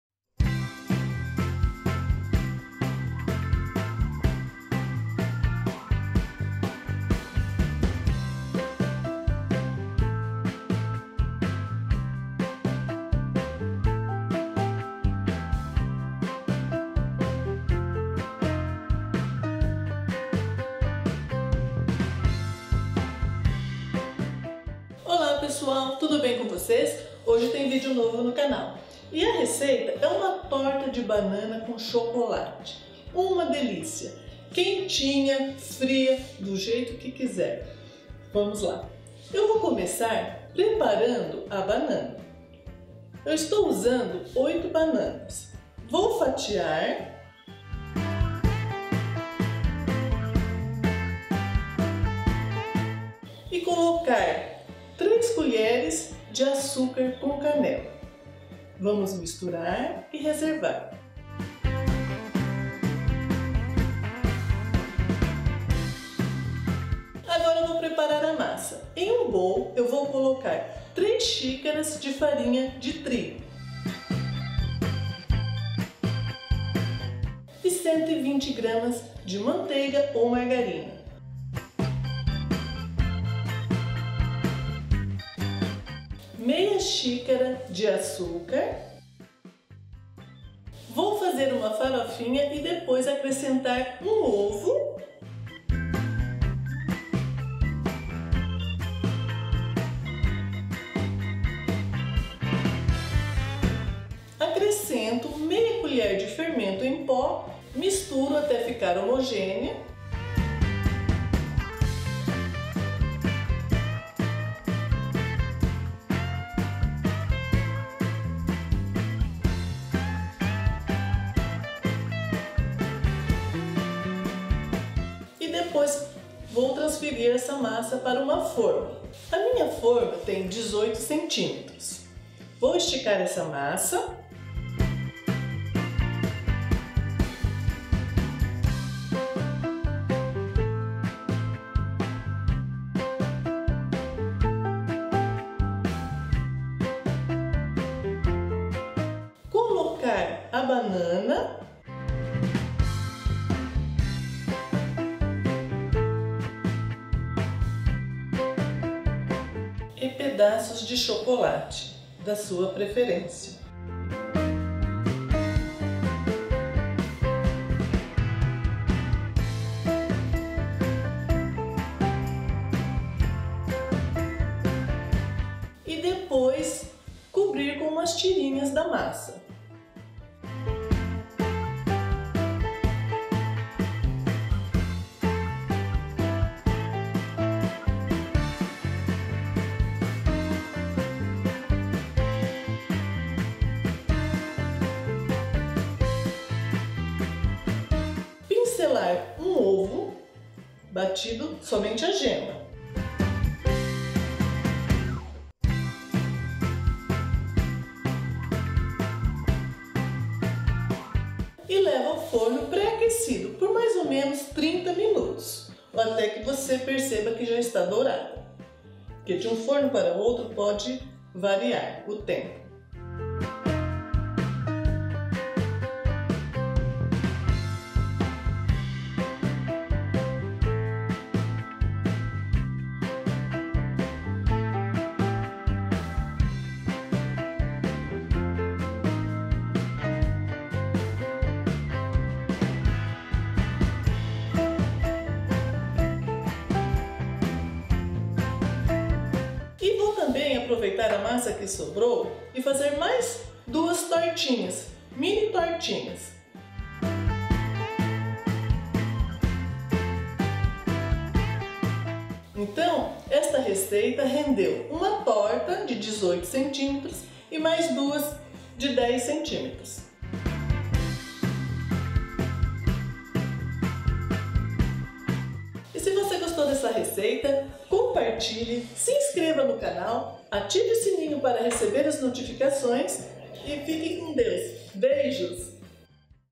Torta de Banana com Canela e Chocolate Simples e Fácil
Aprenda como fazer uma torta de banana com canela e pedaços de chocolate bem simples, fácil e deliciosa.

Recheio:

8 Bananas.
3 Colheres de Açúcar com Canela.

Massa:

3 Xícaras de Farinha de Trigo.
120g de Manteiga ou Margarina.
½ Xícara de Açúcar.
1 Ovo.
½ Colher (sopa) de Fermento em Pó.

Apresentado por Maria Montanher.